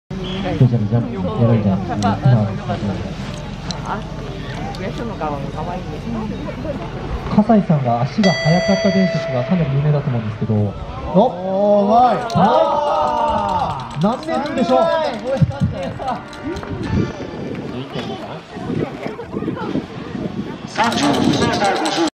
近藤絆 選手 チームタクカップ2021 ２本目 31.5メーター
１本目に３位の絆選手。
この２本目で順位が上がり、最終は小学５,６年男子の部２位。

2021/11/03(水祝) 飯山シャンツェ